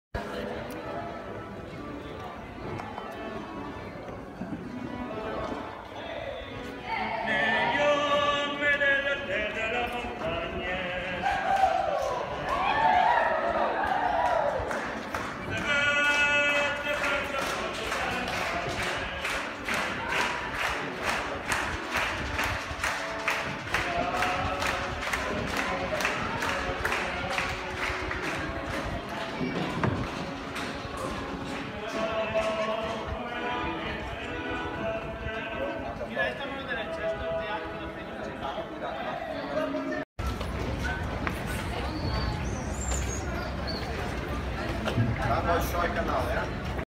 억대 연봉 받는 남자들
베네치아에 곤돌라 뱃사공들의 연봉은 억대로 알려져 있습니다. 그렇다고 아무나 할 수 있는 직업은 아닙니다. 자격 따는 시험도 있고, 명 수도 제한이 있어 경쟁이 치열하죠.